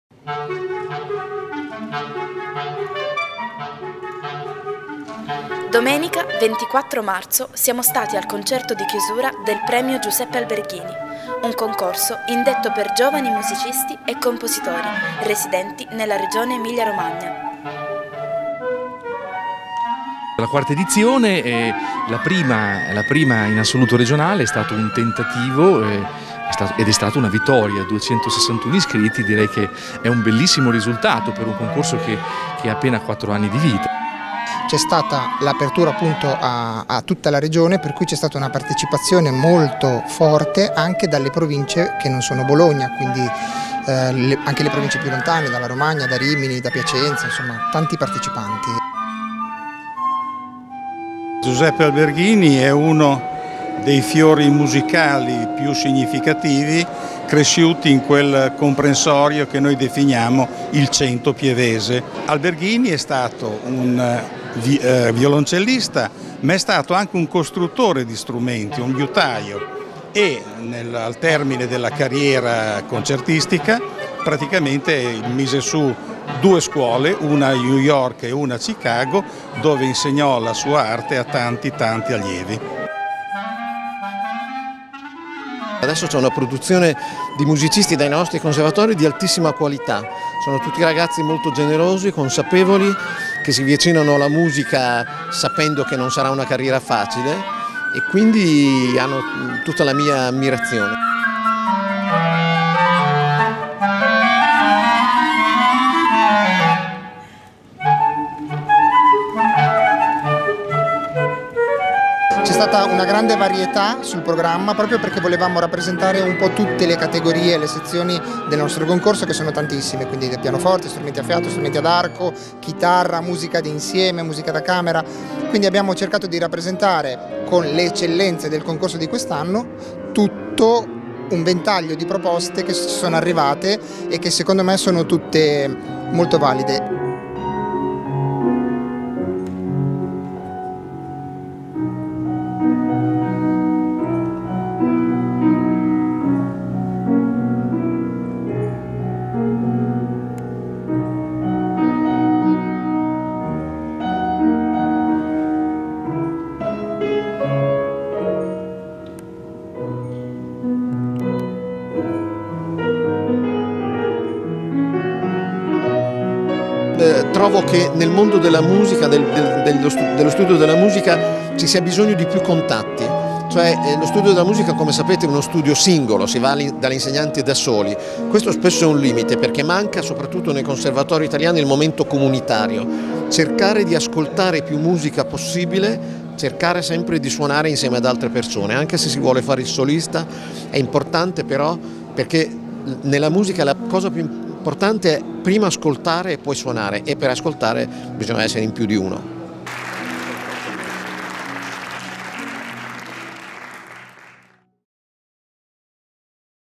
Concerto di chiusura del Premio Alberghini 2019
Domenica 24 marzo 2019 si è tenuto presso il Teatro Comunale il concerto di chiusura del Premio Alberghini, concorso indetto per giovani musicisti e compositori residenti in Emilia-Romagna. Abbiamo intervistato i principali organizzatori.